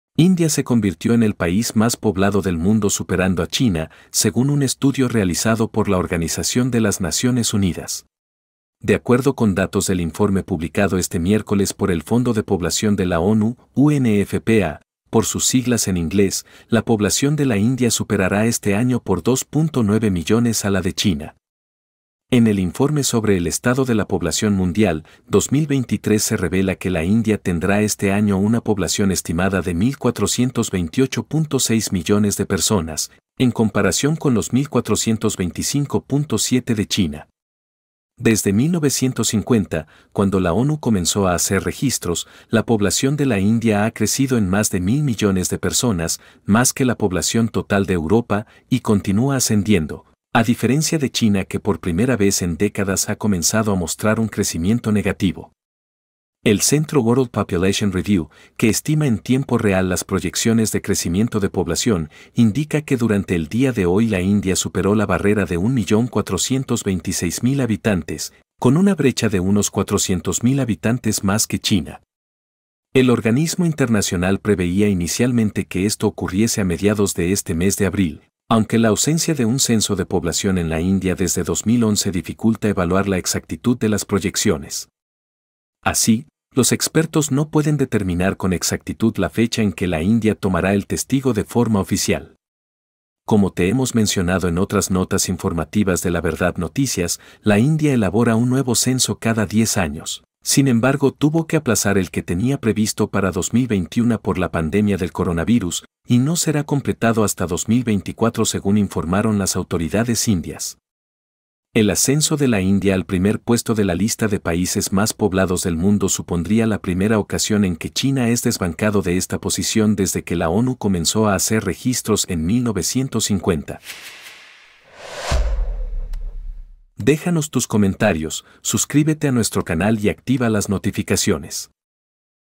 India se convirtió en el país más poblado del mundo: ONU
De acuerdo con datos del informe publicado este miércoles por el Fondo de Población de la ONU (UNFPA, por sus siglas en inglés) la población de la India superará este año por 2.9 millones a la de China.